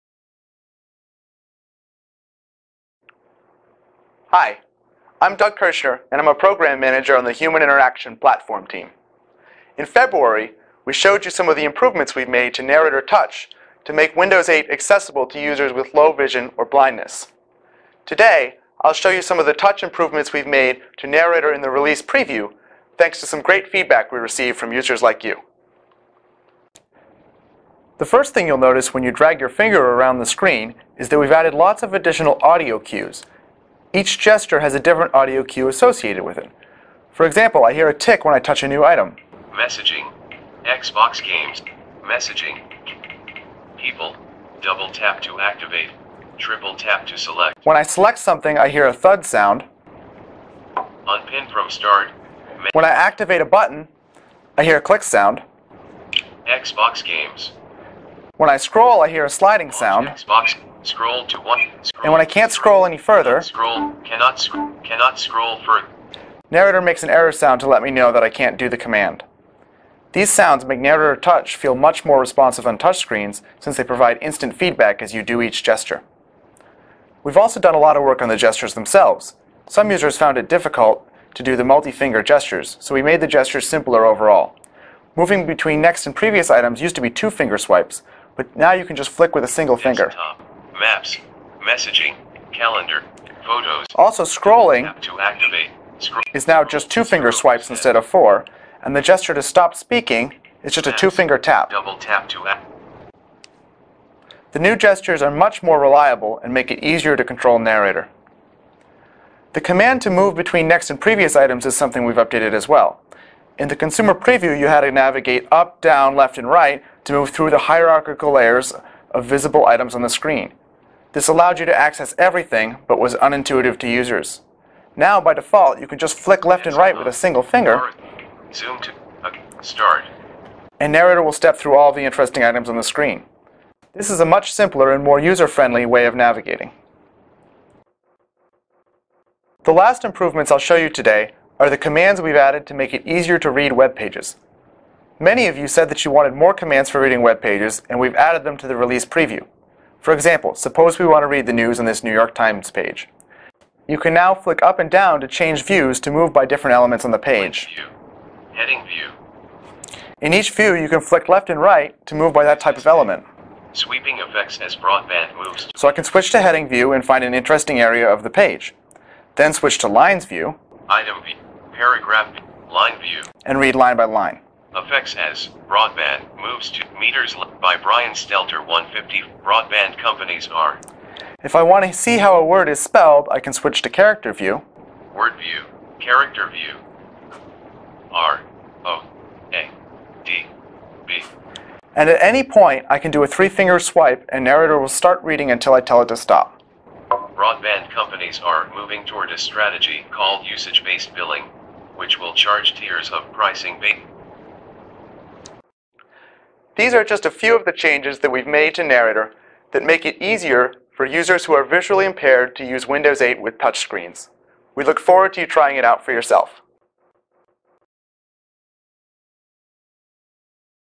Developing Narrator to work better with touch in Windows 8
First off, we would like to thank all the people who have given us feedback; there has been a lot of positive reaction—people are excited that Windows 8 touch screens will include basic screen reading support by default. We've gotten a tremendous amount of constructive feedback on things we could do to make Narrator work better on touch screens and easier to use on the web. We've listened. Your suggestions, combined with suggestions from usability testing on visually impaired users here at Microsoft, have resulted in some important changes that we think you'll really like.